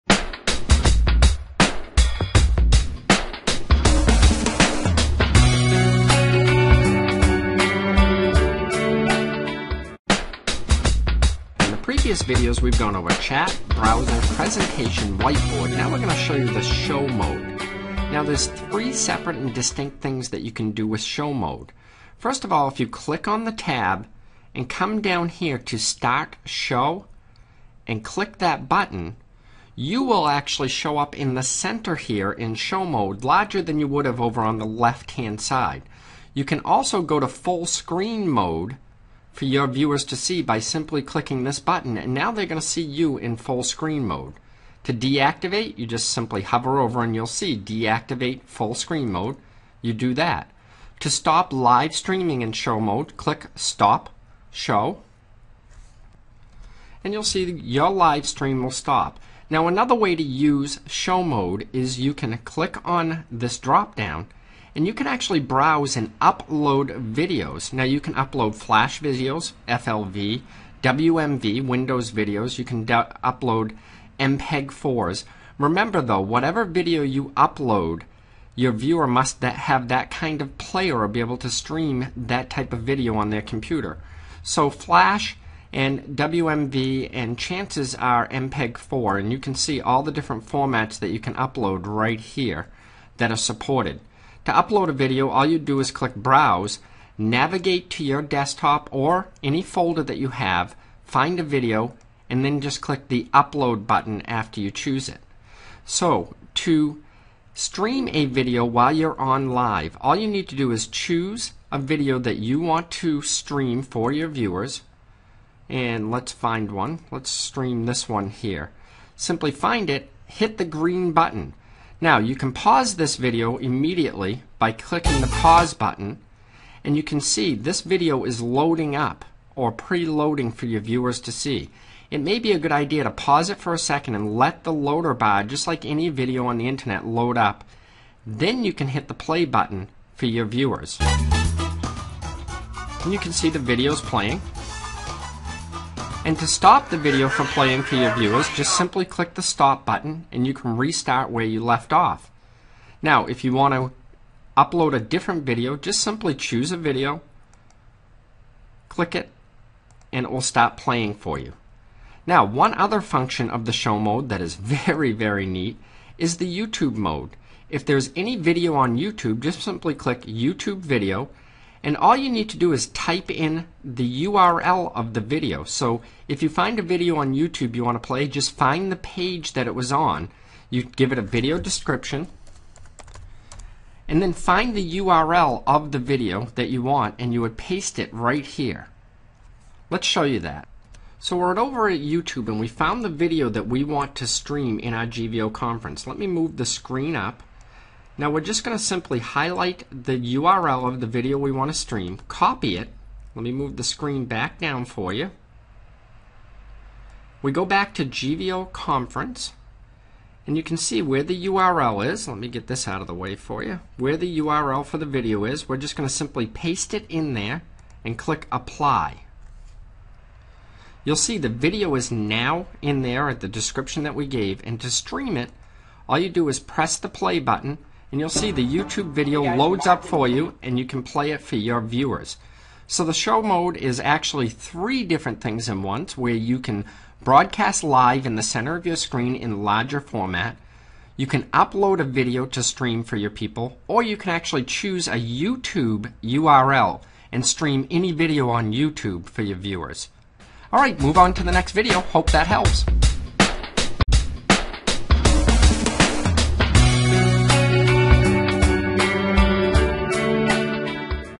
Webcasting Webinar Video Software
How to broadcast, upload video or play YouTube in your 50-Seat  Webinar Conference with GVOConference, use the webinar conference software.  It will Play on any OS Windows, MAC or Linux system.  It will expand to 500 users with Desktop sharing, PowerPoint, Webinar Record, File Transfer, Chat, White Board, and Browser.